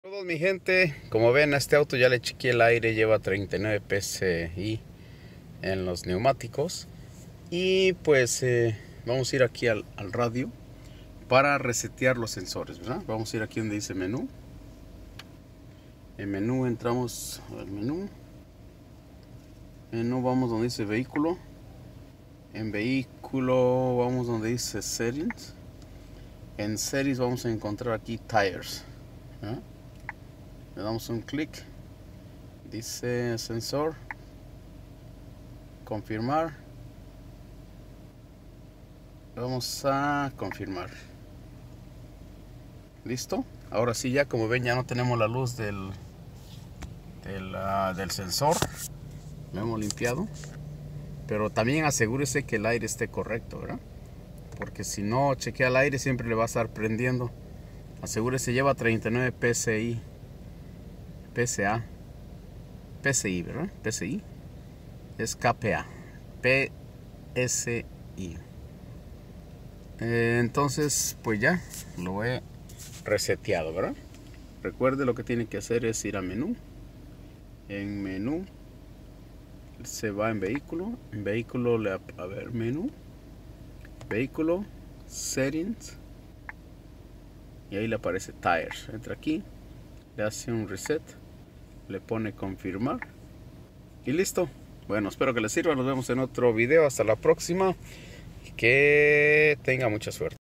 2019 VOLKSWAGEN COM RESETEAR LA LUZ DE LOS SENSORES DE LOS NEUMATICOS FACIL
RECUERDE PRIMERO AJUSTAR EL AIRE   TAL VEZ TENGA DE MAS  O MENOS    LLEVA 39 PSI  EL CADA UNA  Y DESPUES RESETEE  LOS SENSORES